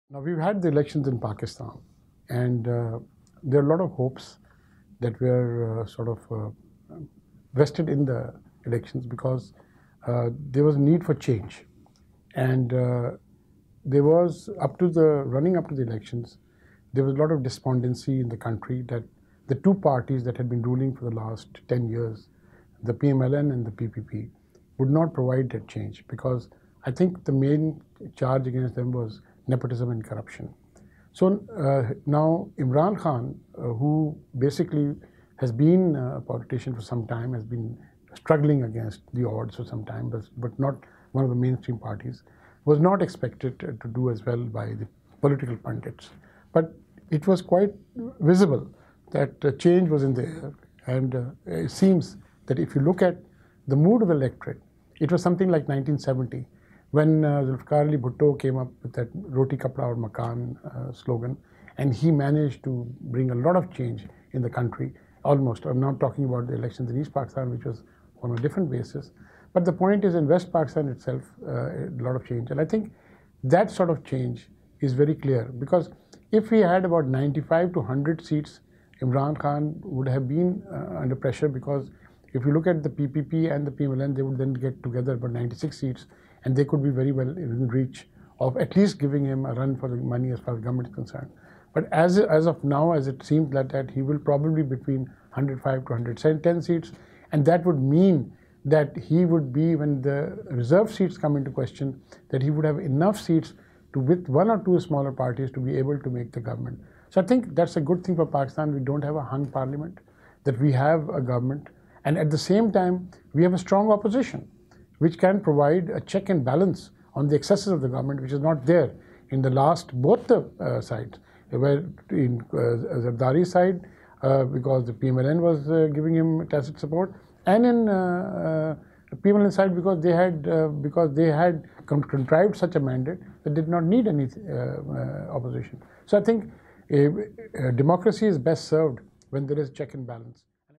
Ikram Sehgal: Pakistan Elections
Ikram Sehgal, chairman of Pathfinder Group reacts to the most recent elections in Pakistan.